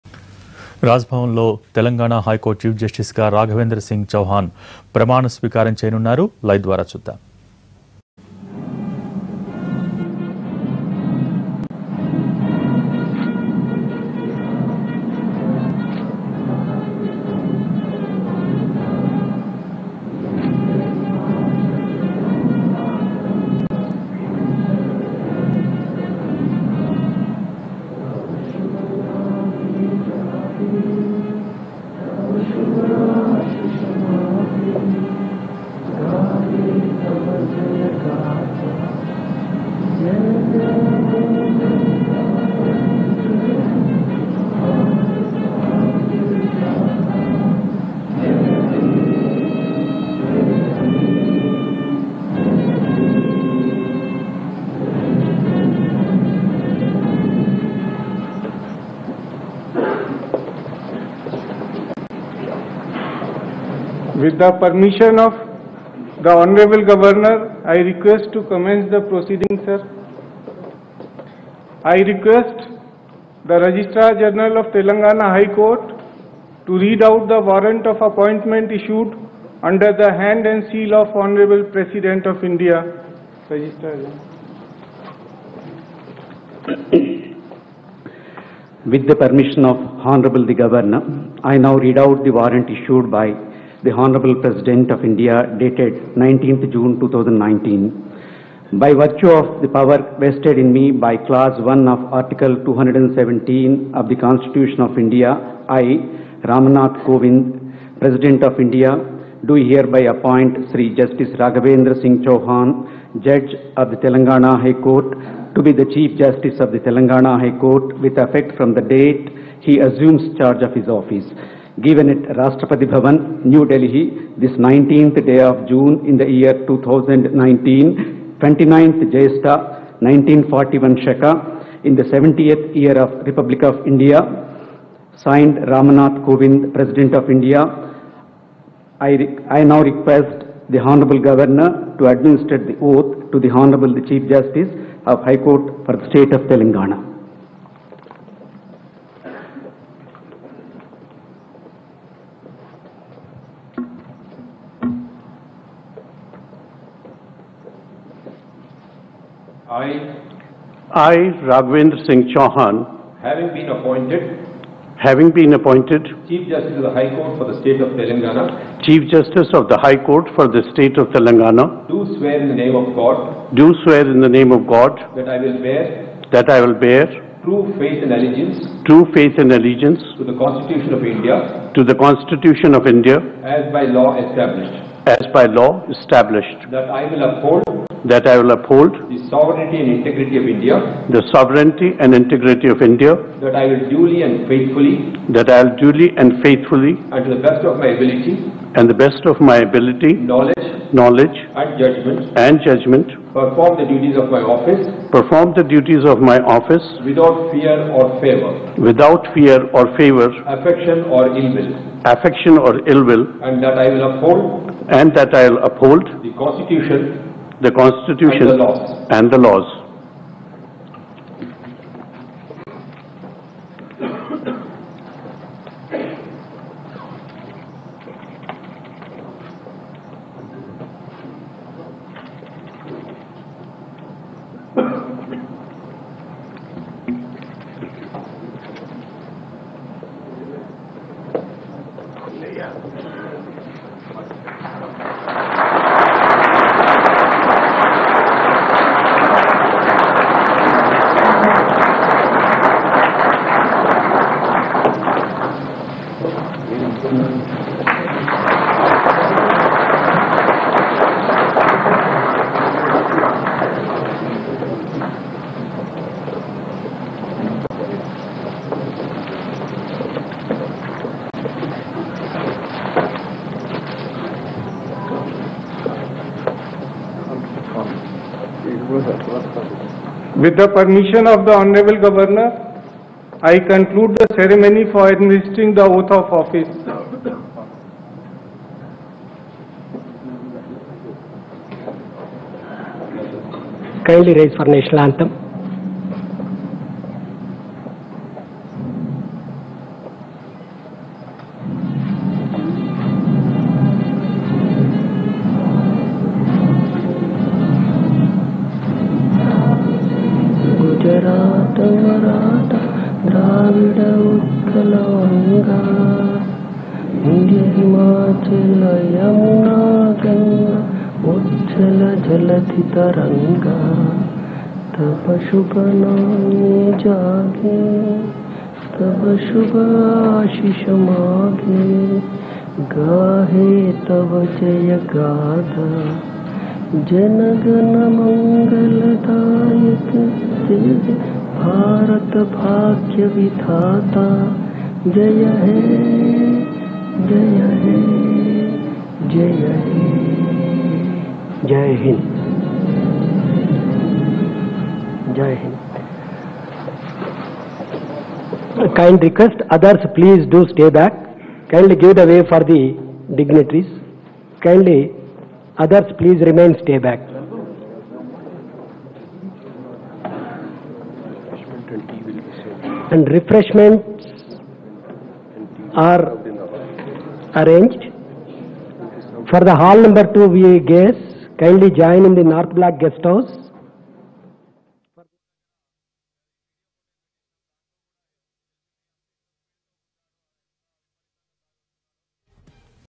Raghavendra Singh Chauhan Sworn In As Telangana HC Chief Justice At Raj Bhavan || NTV
Raghavendra Singh Chauhan Sworn In As Telangana High Court Chief Justice At Raj Bhavan